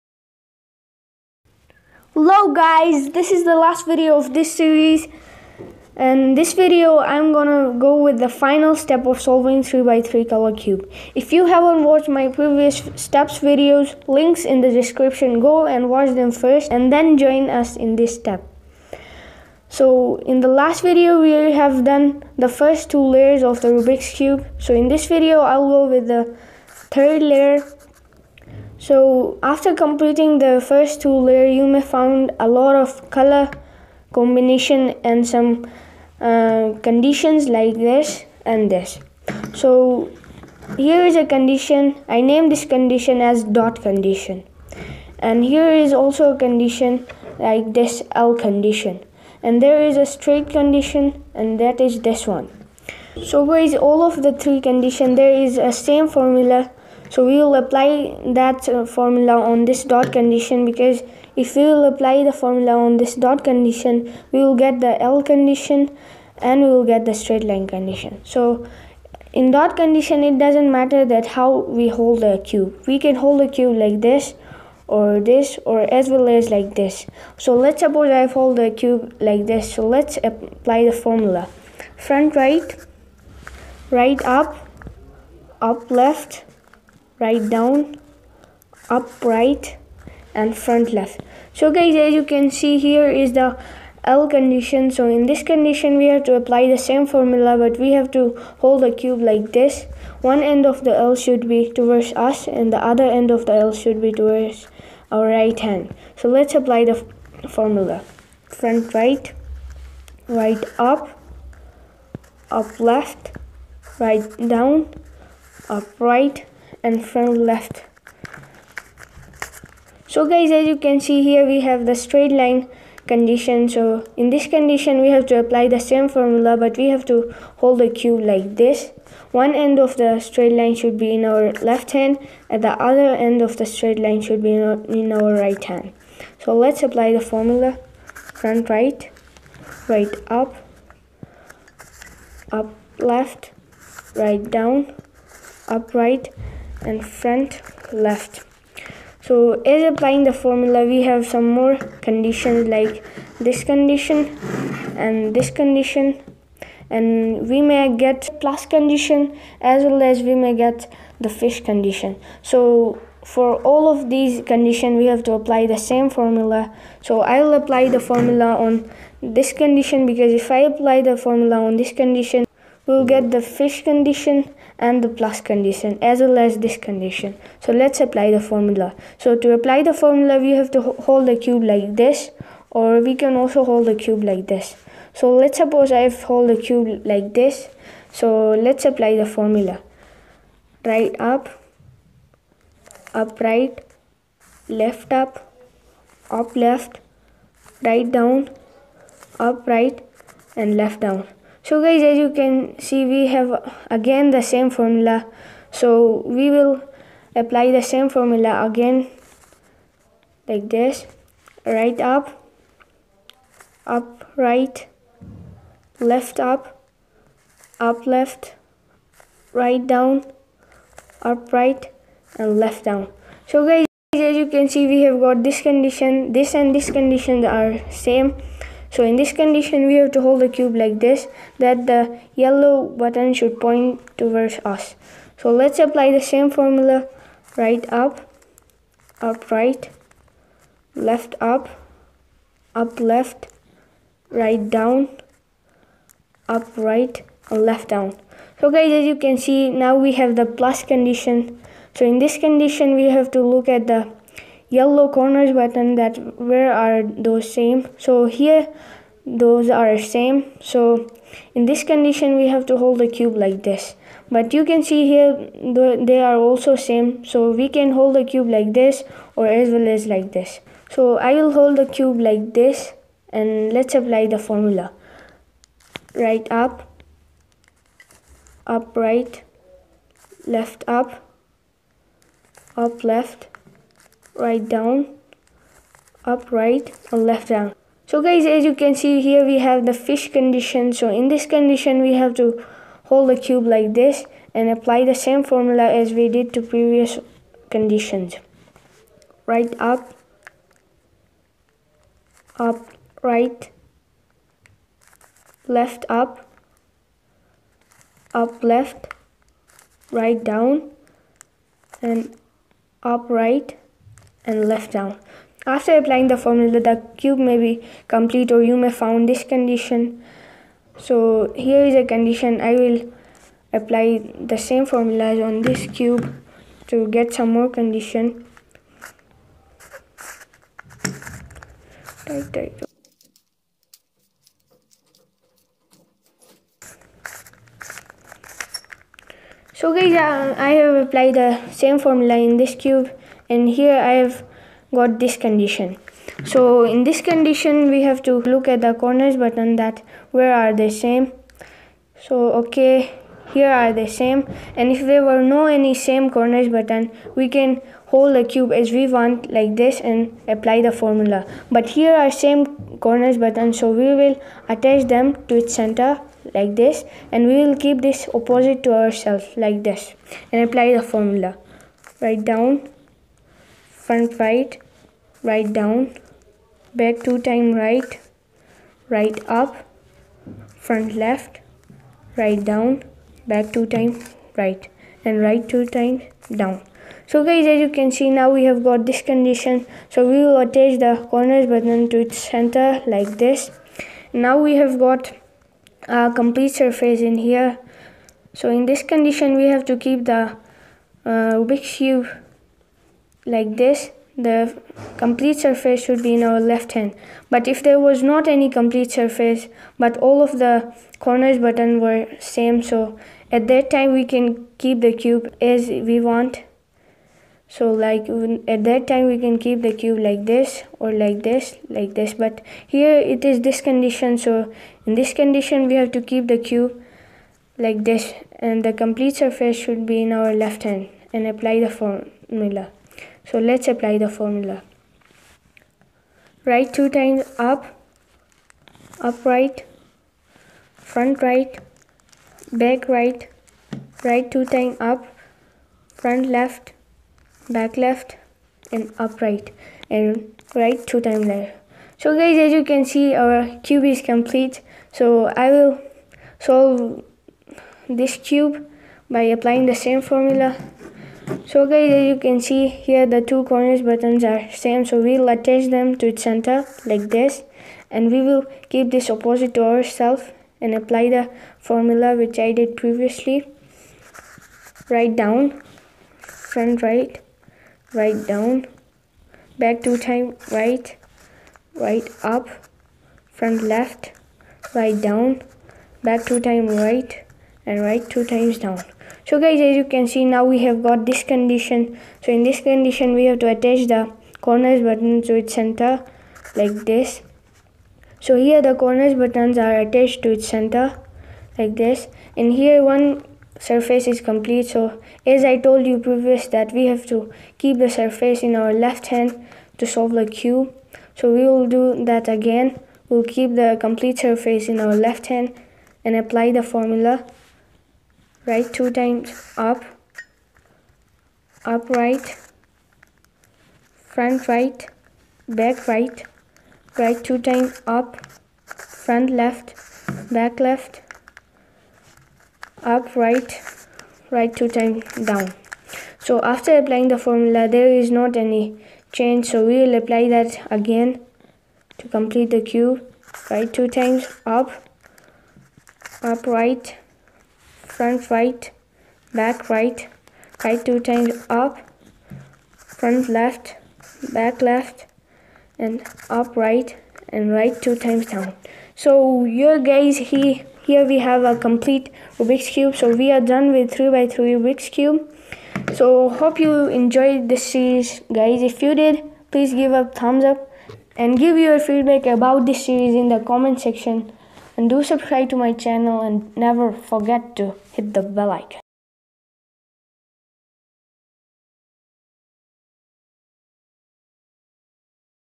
How to solve 3x3 Rubik's Cube| Final step| Easiest Tutorial
Hello viewers,

This series is for the very beginners to learn how to solve 3x3 color cube (Rubik’s Cube). It’s the easiest way for the beginners to solve the cube in 4 steps. The method I teach in this series is featured with a simple layer by layer approach and it’s the most basic and simple approach to solve the cube.

To make the tutorial easy for you guys, I’ve divided the whole process of solving the cube in 4 easy and simple steps. For each step I’ve made short separate videos. You need to go through each video to solve the cube.

This the final video of the series solving the cube. Its 4th & final step: Completing the Rubik’s cube.  The previous videos of this series are on my YouTube channel. Go and watch the videos first, in order to solve the cube properly.